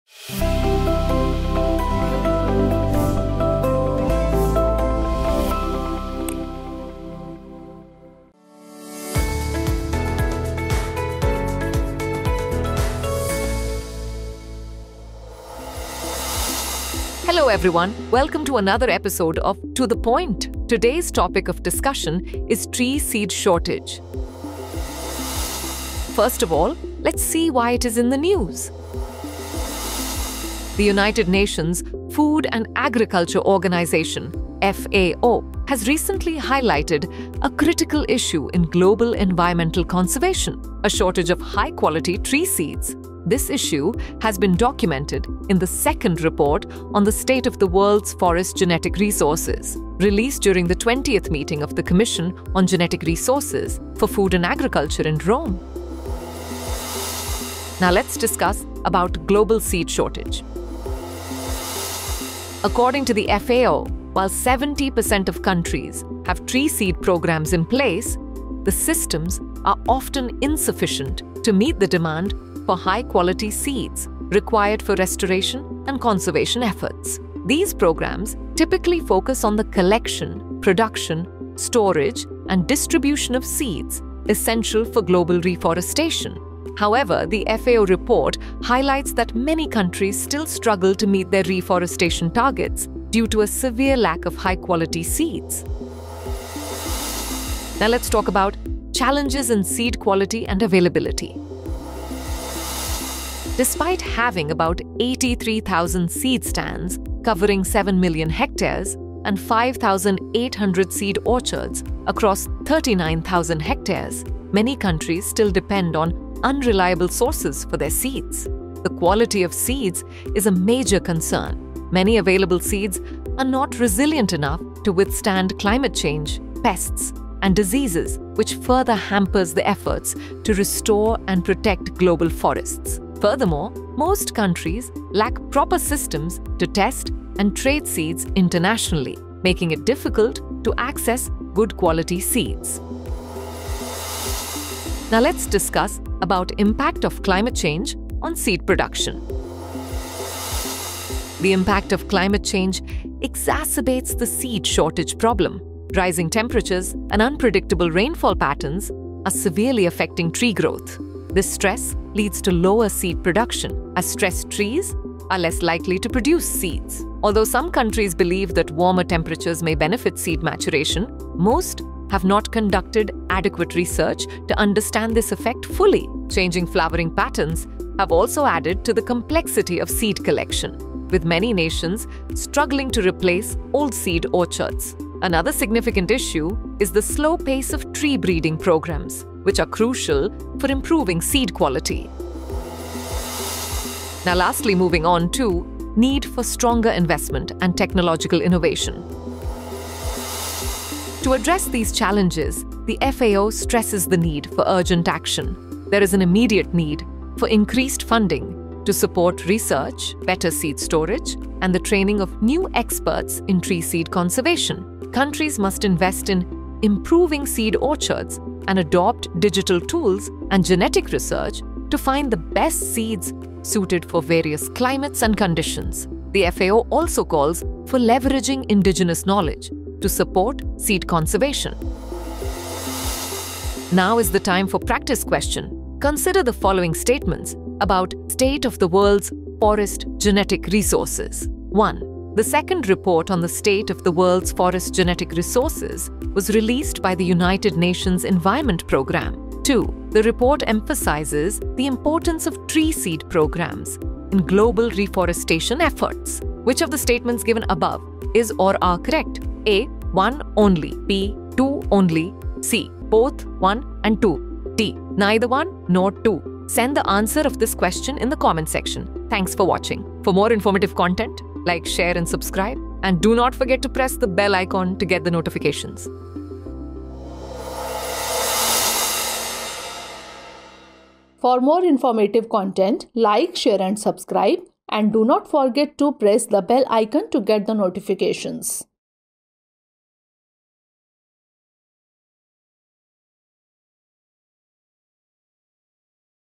Global Seed Shortage | FAO Report | To The Point | Drishti IAS English
About Global Seed Shortage

Challenges in Seed Quality and Availability

Impact of Climate Change on Seed Production

Need for Stronger Investment and Technological Innovation